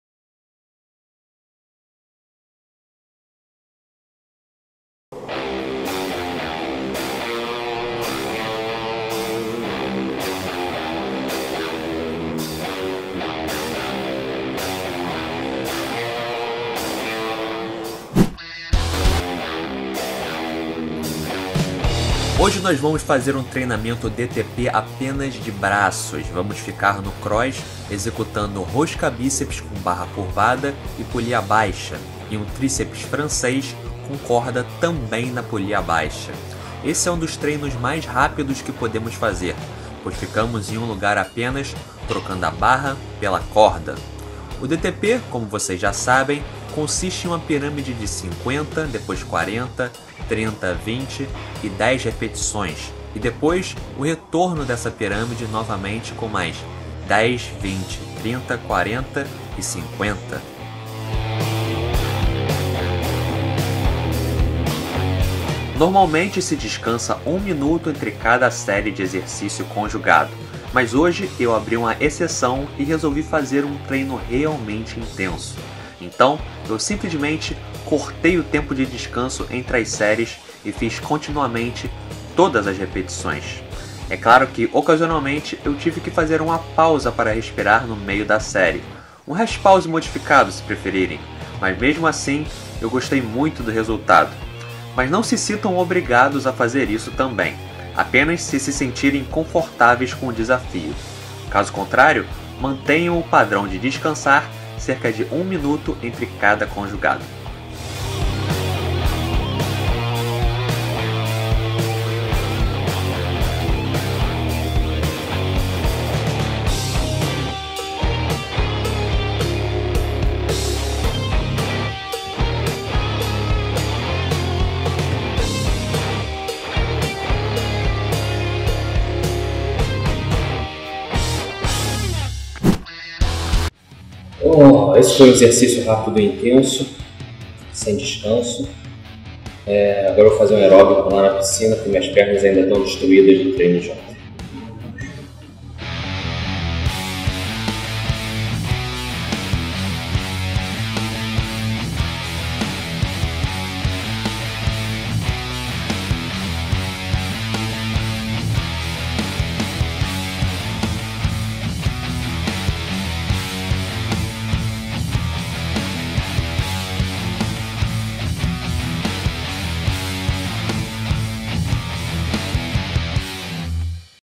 Treino de braços: DTP | Transformação corporal em 12 semanas - Dia 75
Hoje vamos aplicar o DTP aos braços. É um treino rápido e eficiente e se estiverem se sentindo confiantes, podem experimentar a variação que eu fiz no vídeo.